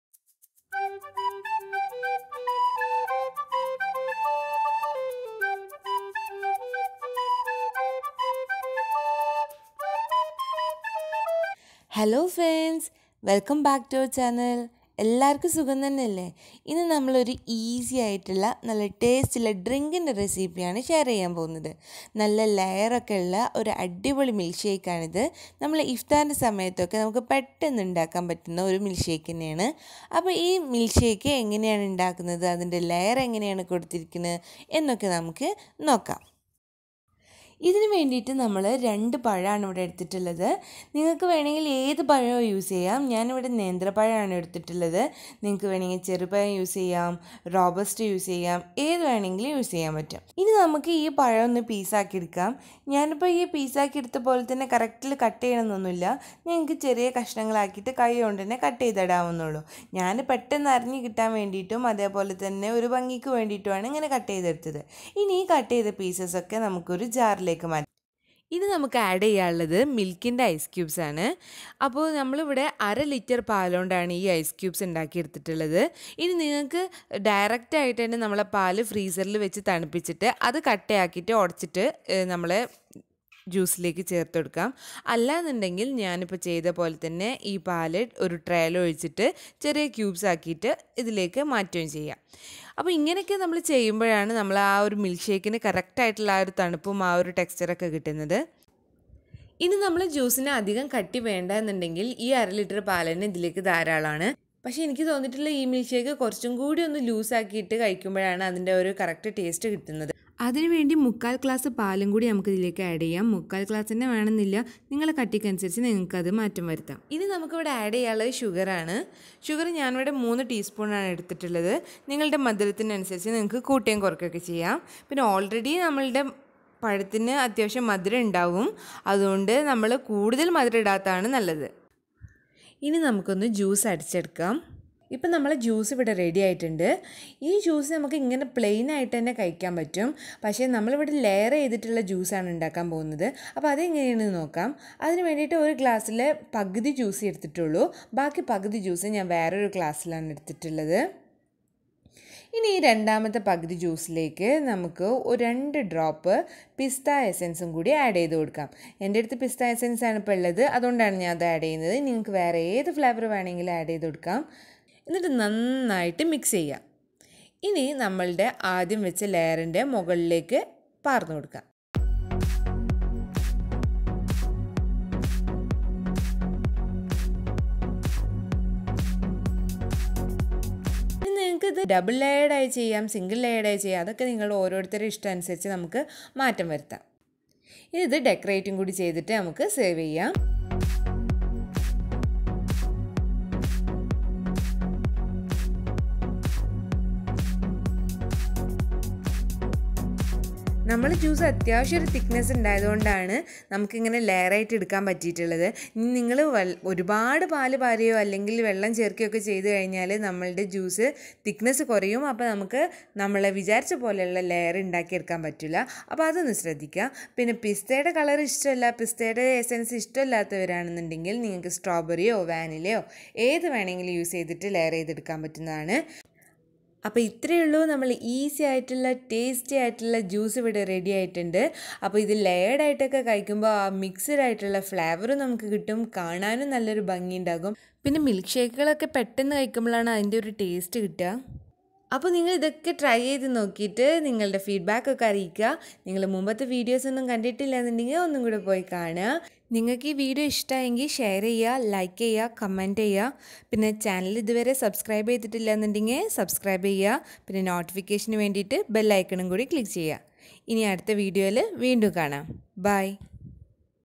കൊതിയൂറും Simple Layered milkshake /banana dessert/ifthar drink 2020/banana juice /ramalan reciepe
banana layered milkshake

ingredience

banana
milk
suger
 pista essence for layering

it is a cold drinks. so it will chill during ifthar time.

ifthar drink2020
ramalan reciepe
easy juice